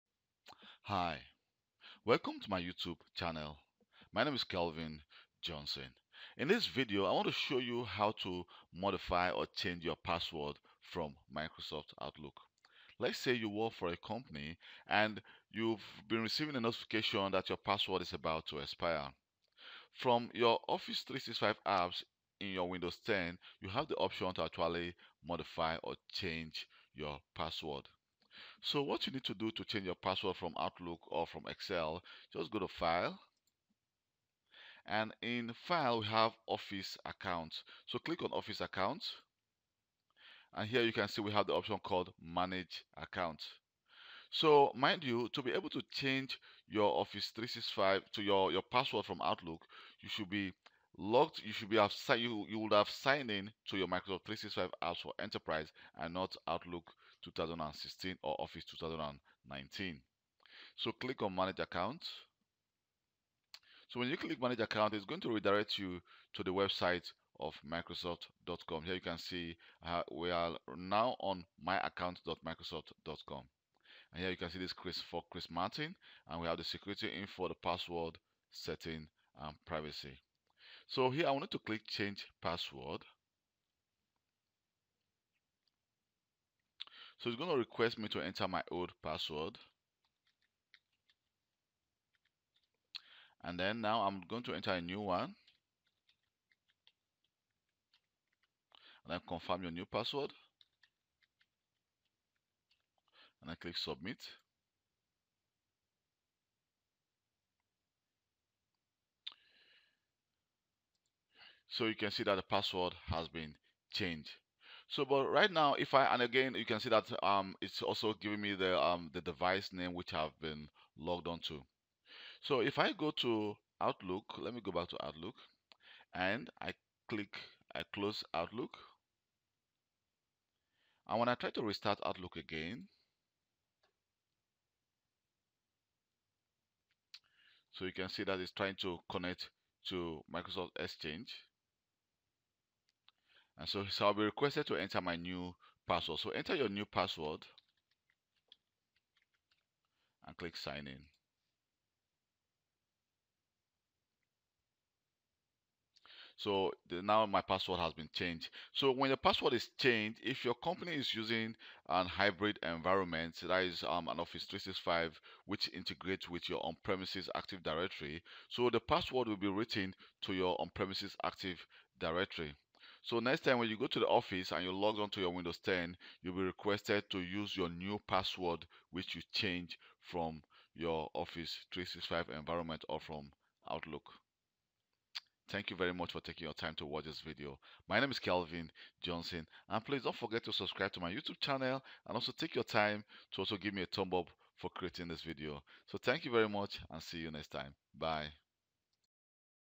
How to Change Your Password in Outlook 365 and Sync it to Microsoft 365 Cloud | Sync outlook Pwd
This video explain how to Change Your Password from Microsoft 365 Apps Outlook and Sync it to your company system.